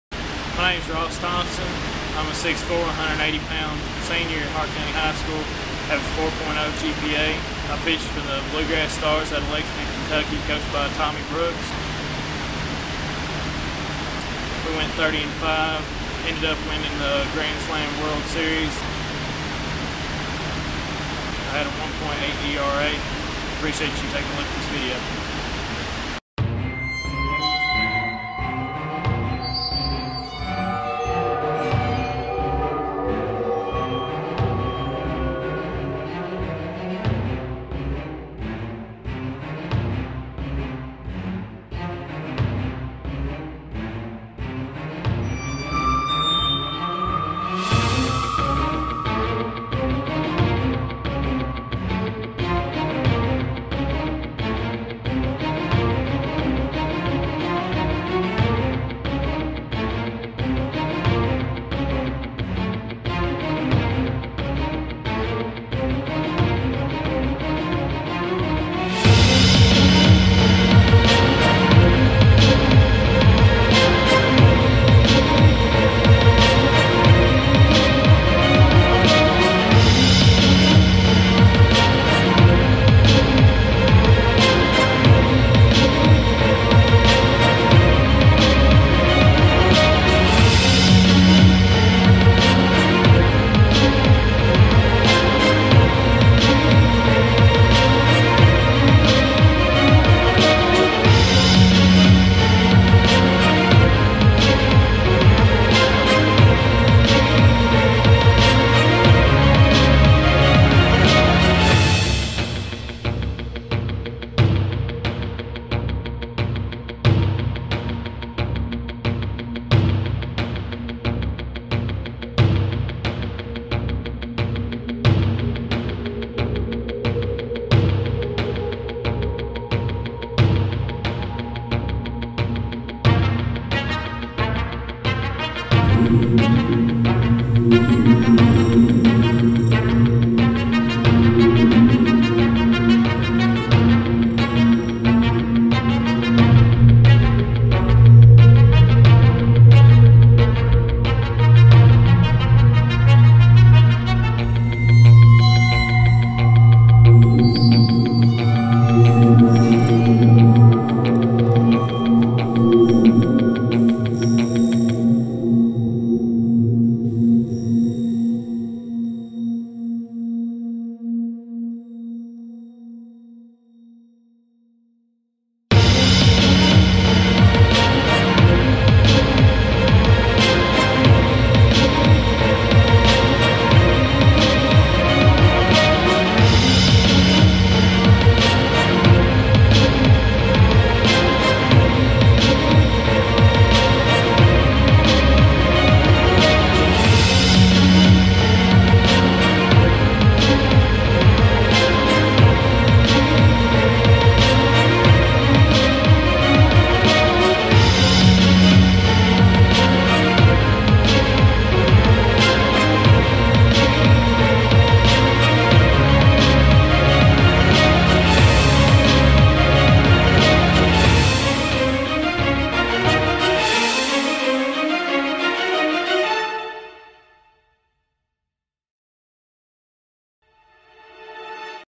Ross Thompson Fall 2014 Pitching Video
Ross Thompson is a Senior at Hart County High School in Munfordville, KY. He currently plays on the Hart County High School Baseball and Basketball teams as well as the "Bluegrass Stars" from Lexington, KY.

Closed Captioning is available for this video.

Video was created by Jarrett Cox